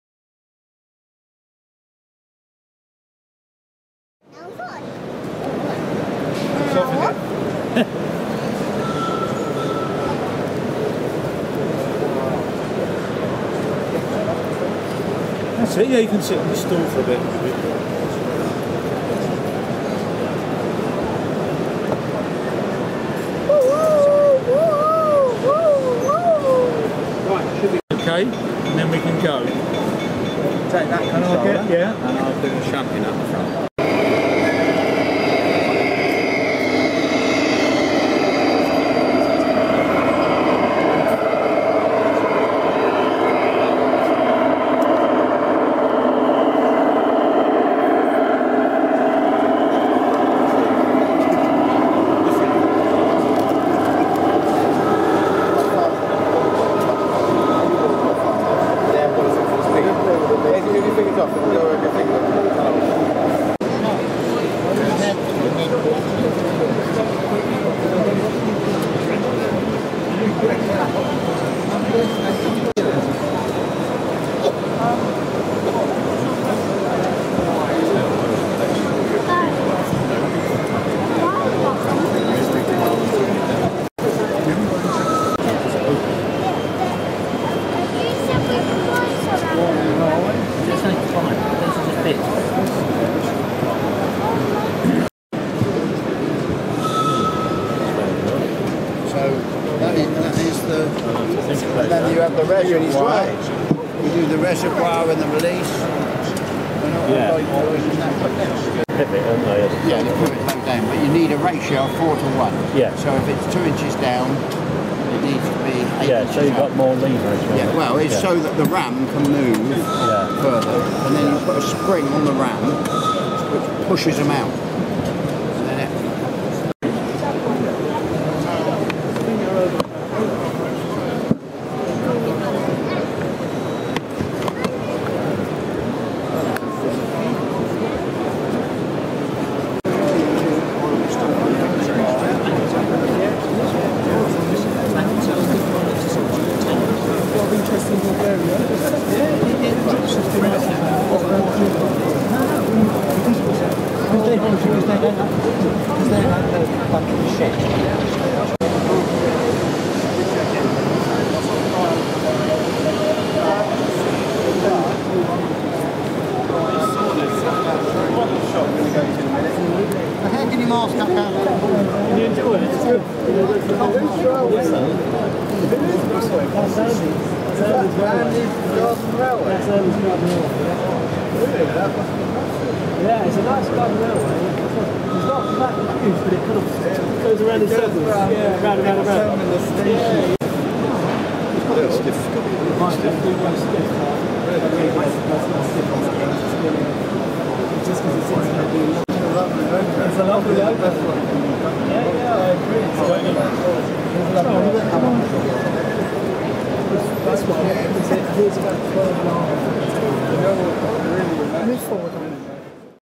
London Model Engineeting Exhibition 2011
I had a very limited time this year, hence the short video. See some of the new PIKO models: Harz Kamel, BR 204, PNP Railway products for garden railways, Maxitrak stand with the highly detailes Class 20